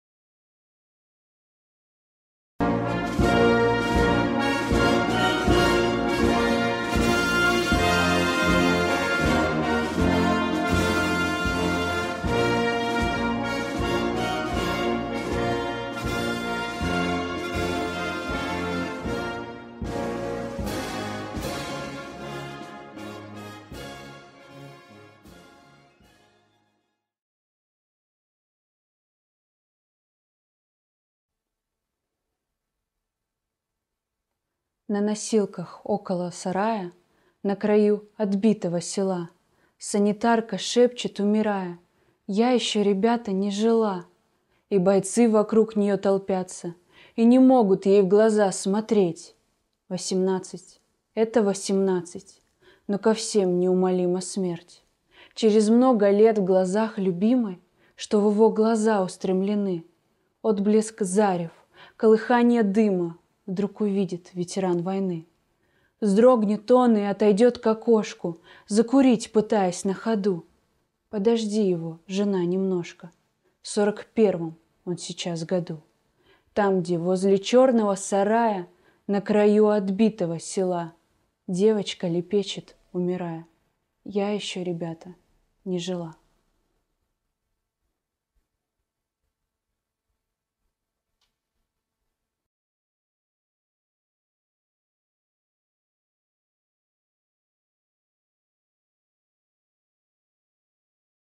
Юлия Друнина, "На носилках, около сарая…". Читает Полина Соловьева, г.п. Белый Яр Сургутского района
Юлия Друнина, "На носилках, около сарая…".
Читает Полина Соловьева, г.п. Белый Яр Сургутского  района Ханты-Мансийского автономного округа — Югры

Юлия Владимировна Друнина (10 мая 1924 — 21 ноября 1991) — советский поэт. Член Союза писателей СССР. Секретарь Союза писателей СССР и Союза писателей РСФСР.
В 1942 году, в числе девушек-медиков, была направлена в санитарное управление 2-го Белорусского фронта, получила назначение в 667-й стрелковый полк 218-й стрелковой дивизии. В 1943 году была тяжело ранена. После излечения вернулась на фронт, попала в 1038-й самоходный артиллерийский полк 3-го Прибалтийского фронта. Воевала в Псковской области, затем в Прибалтике. В одном из боёв была контужена и 21 ноября 1944 года признана негодной к несению военной службы.